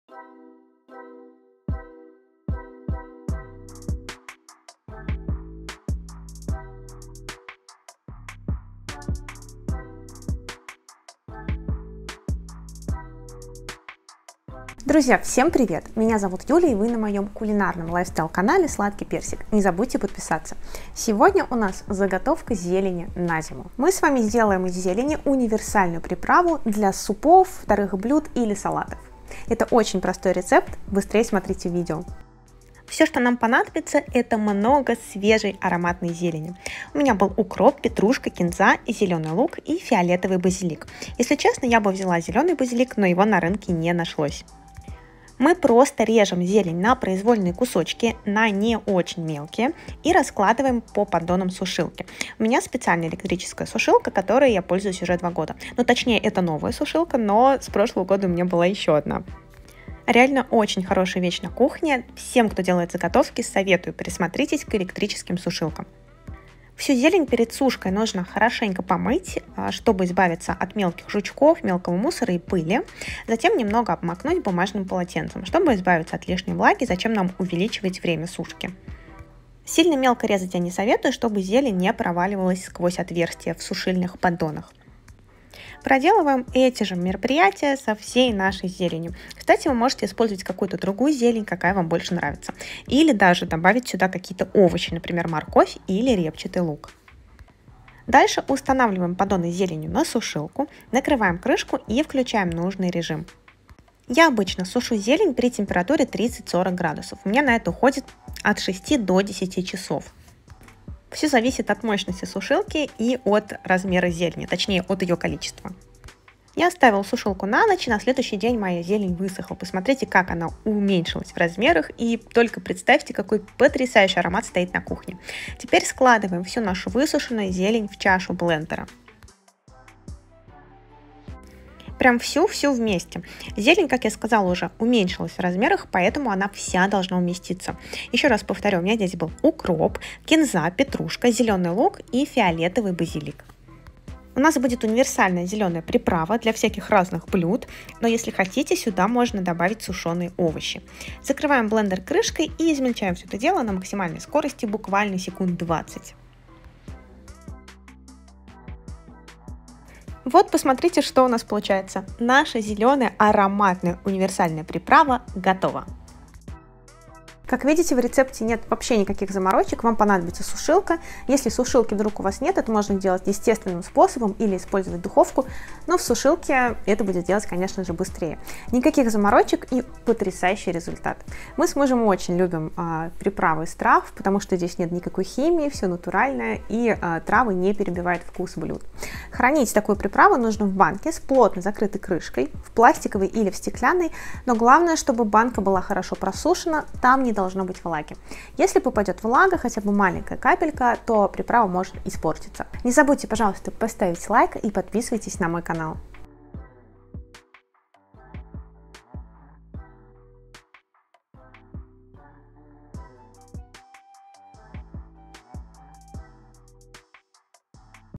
Зелень на зиму | Самый лучший и универсальный способ заготовки!
Как заготовить зелень на зиму? Что лучше – сушка зелени или заморозка?
Показываю классную универсальную приправу для ВСЕХ блюд. Из свежей зелени!
Много места не занимает, использовать легко!